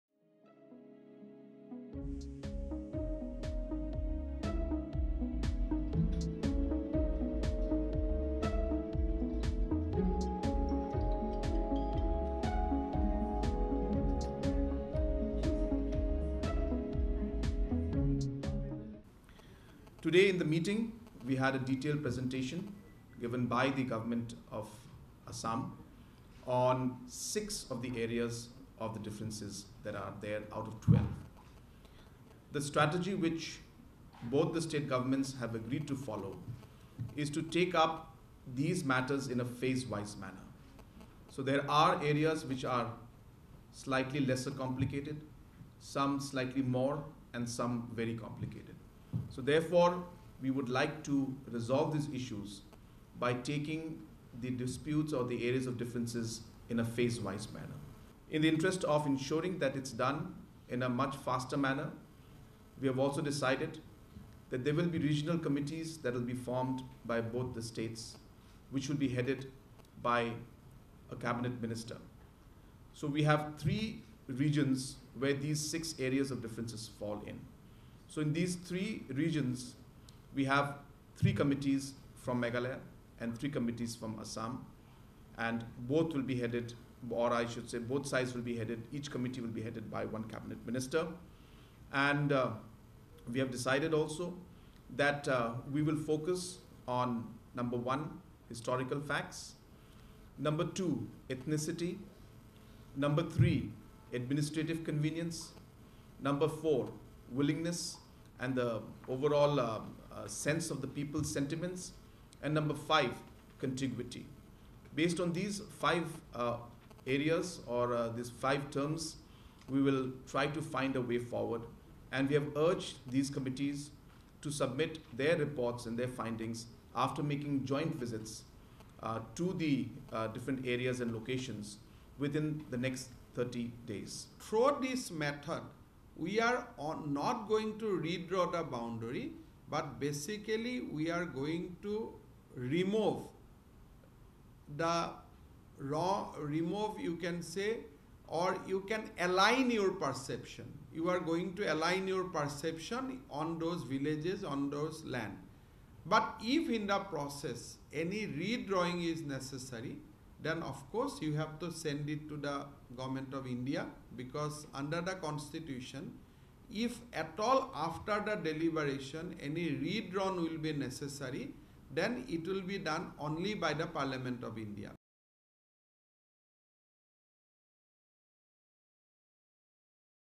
Border dispute: Assam, Meghalaya CMs hold 2nd round of border talks
Guwahati (Assam), Aug 06 (ANI): Chief Ministers of Meghalaya and Assam held the second round of talks over the inter-state border dispute on August 06. The meeting held in Guwahati. Meghalaya Chief Minister Conrad Sangma said, “Today in the meeting, we had a detailed presentation given by the Assam govt on six out of twelve areas of dispute. Both states have decided to form regional committees which will be headed by Cabinet Ministers”. He also added, “We've three regions where these six areas of differences fall in. 3 committees from Meghalaya and 3 from Assam will focus on historical facts, ethnicity, administrative convenience, willingness, and contiguity and submit their reports within 30 days.” Assam Chief Minister Himanta Biswa Sarma said, “Through this method, we're not going to redraw the boundary but change the perception about those areas or villages. If redrawing of boundary is necessary, we will recommend that to Parliament.” (JA)